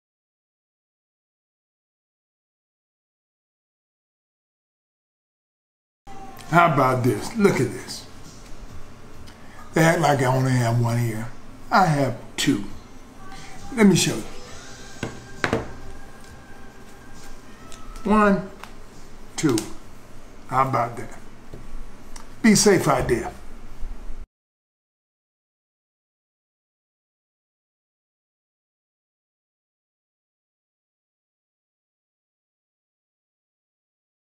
EVANDER HOLYFIELD REACTS TO MIKE TYSON EAR MEME! LOL! GIVES HILARIOUS RESPONSE!
Evander Holyfield gives a hilarious response to the mike tyson ear meme that has circulated over the internet about him as a result from the mike tyson vs evander holyfield 2 fight as he sends a message to people about staying safe!

EVANDER HOLYFIELD REACTS TO EAR MEME! LOL! GIVES HILARIOUS RESPONSE!